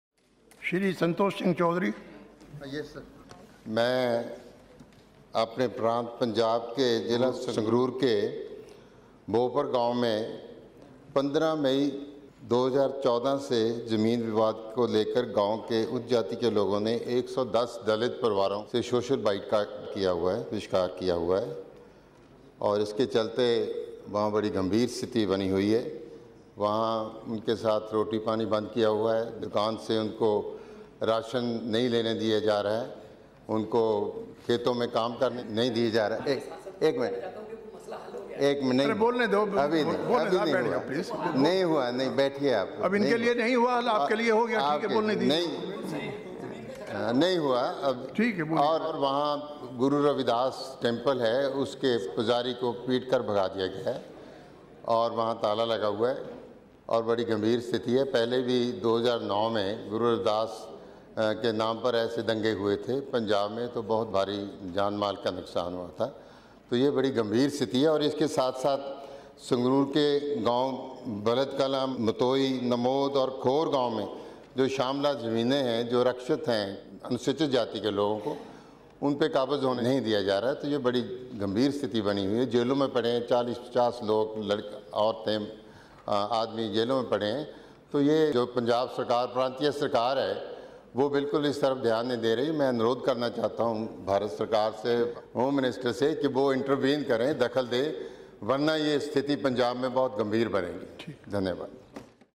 Santokh Singh Chaudhary~ On the matter of land dispute in a village near Sangrur
Throwing light on the atrocities committed by the local families on Dalits and cutting down their supplies of ration creating thoroughly distressful living conditions.